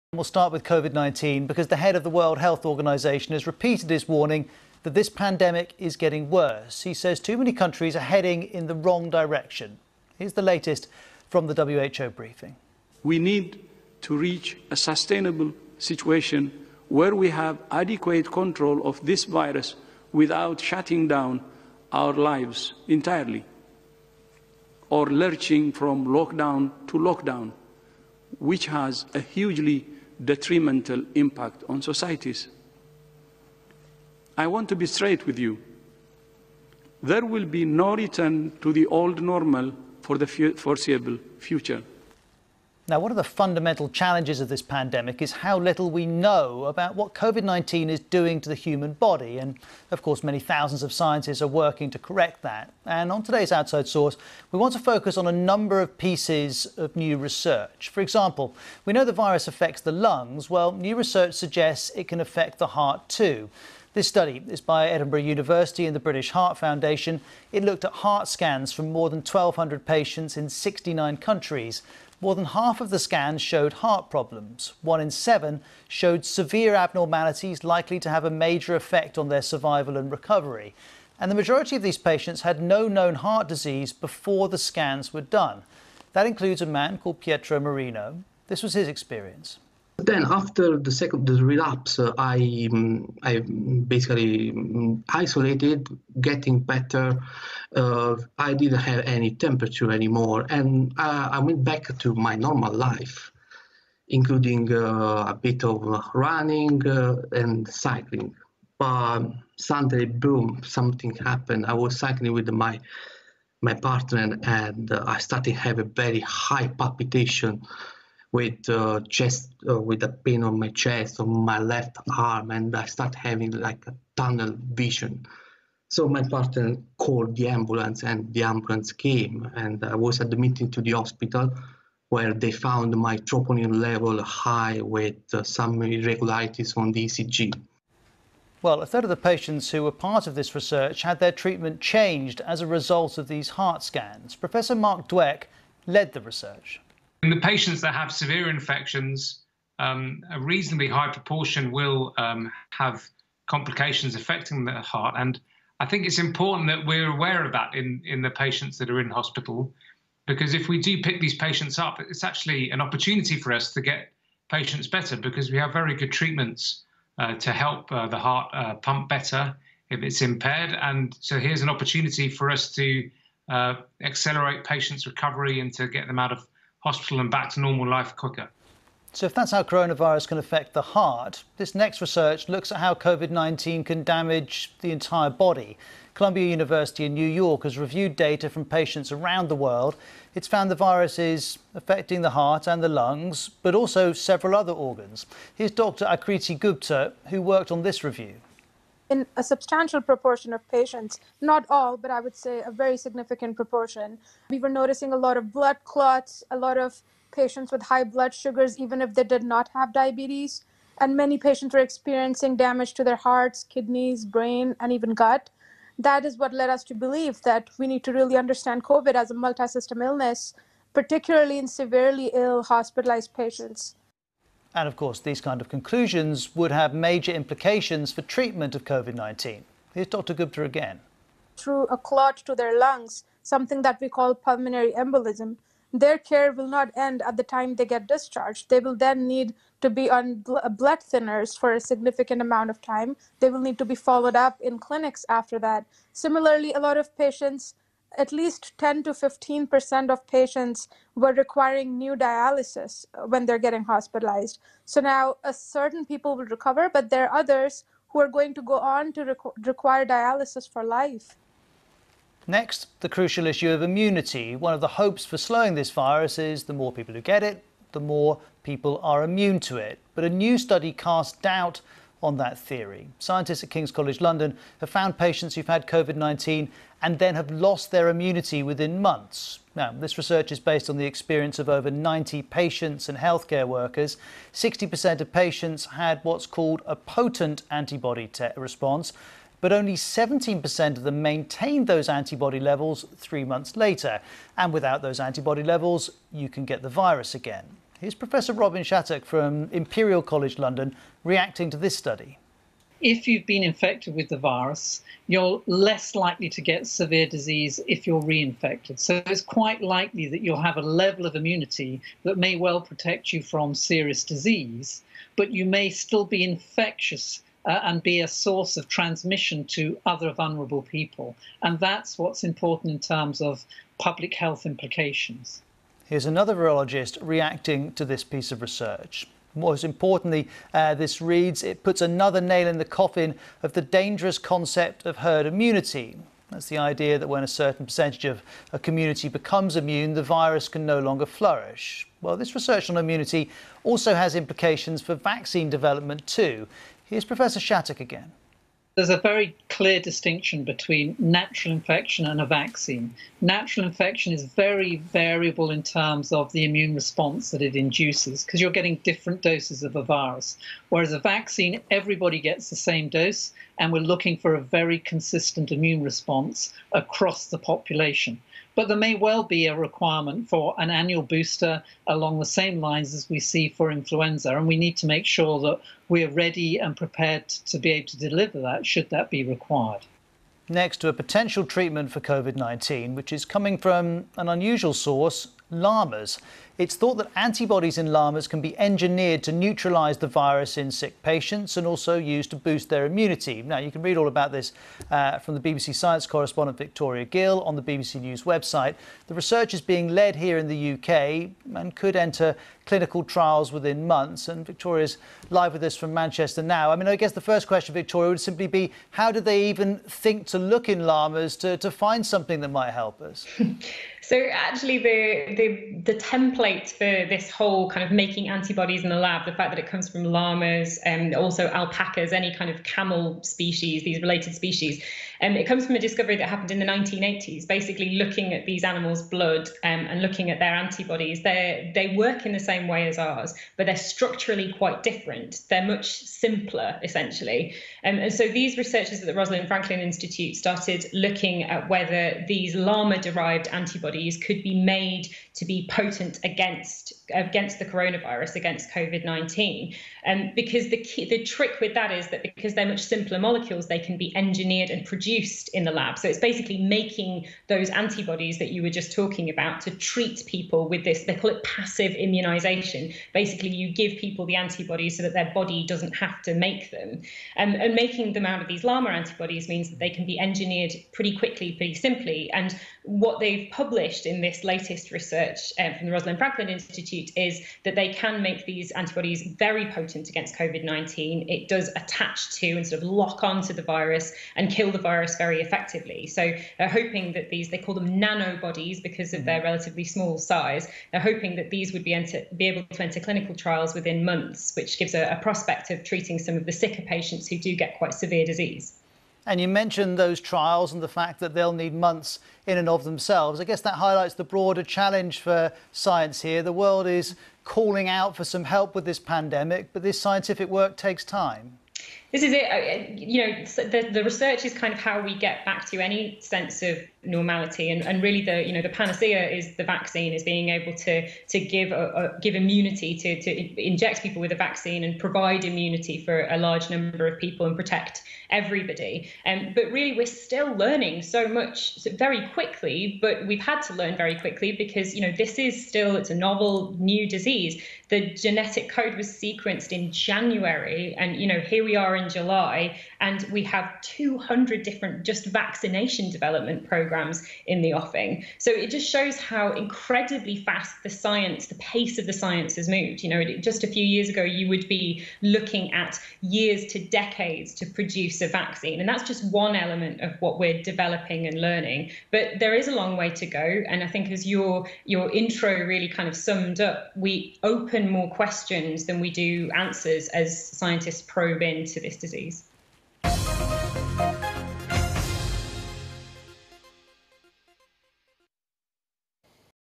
Coronavirus: Nations heading in wrong direction with Covid-19, says WHO
The coronavirus pandemic will get "worse and worse" if governments fail to take more decisive action, the World Health Organization (WHO) has warned. Director general Dr Tedros Adhanom Ghebreyesus said "too many countries [were] headed in the wrong direction". Cases were rising where proven measures were not adopted or followed, he added. The Americas are the current epicentre of the pandemic. The US has seen a rise in cases amid tensions between health experts and President Donald Trump.